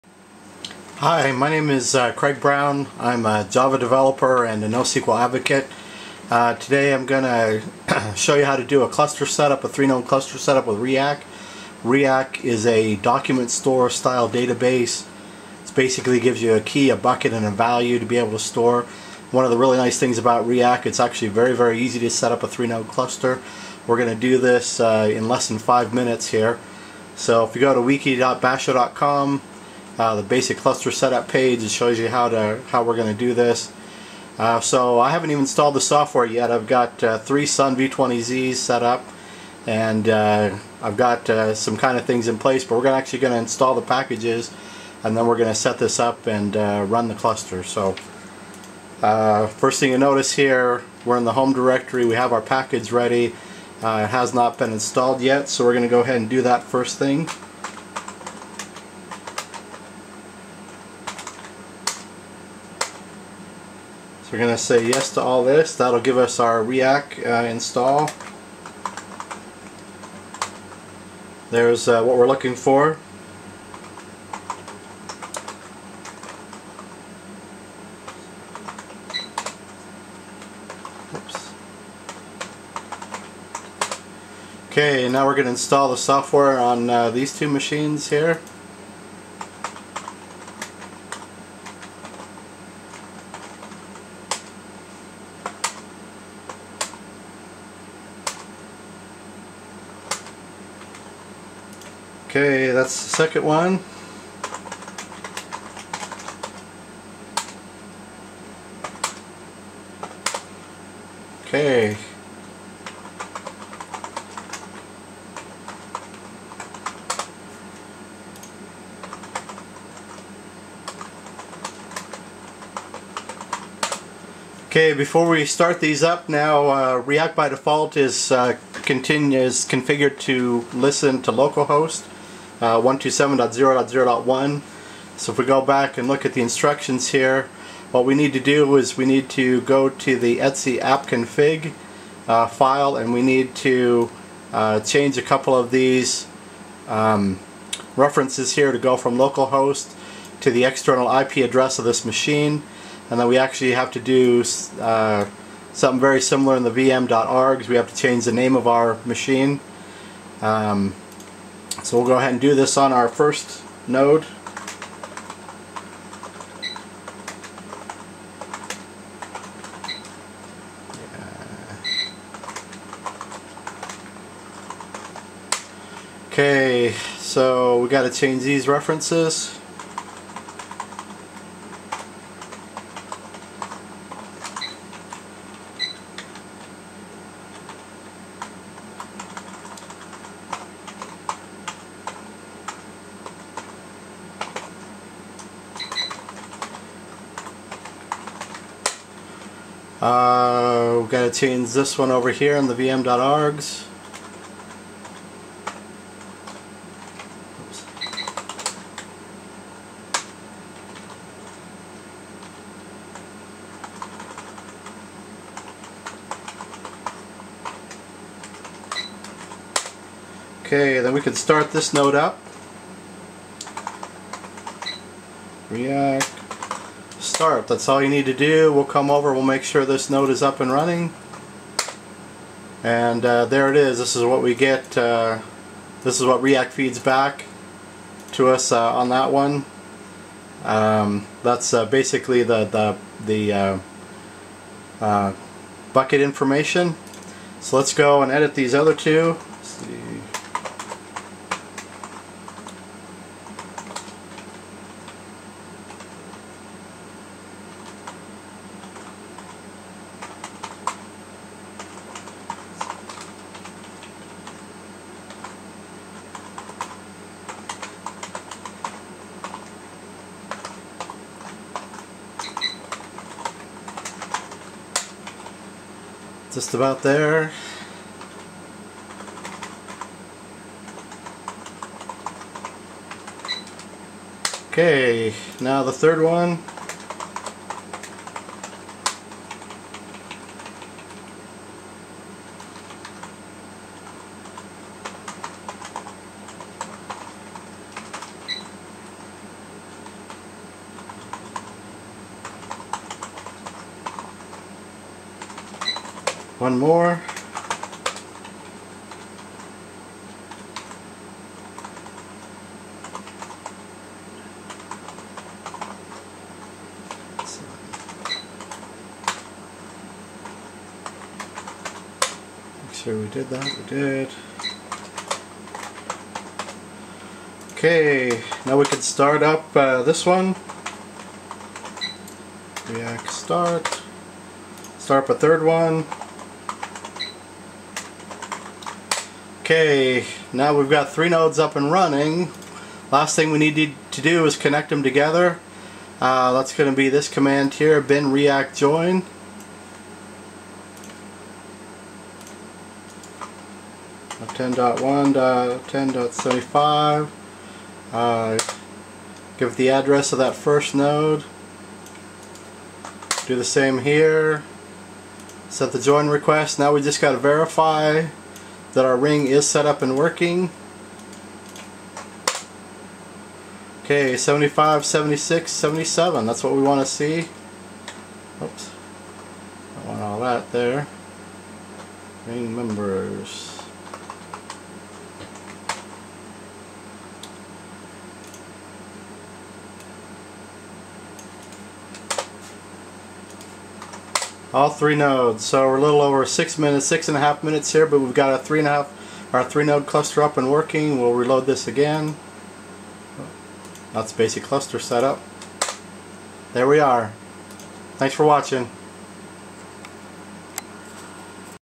Riak cluster in 7 minutes
This video demonstrates the setting up of a 3-node Riak cluster. It took me just less than 7 minutes, I was hoping for 5 minutes or less. This includes installing the software, editing config files, starting up and testing all 3 nodes. File quality is not the best, but I hope to improve that in future videos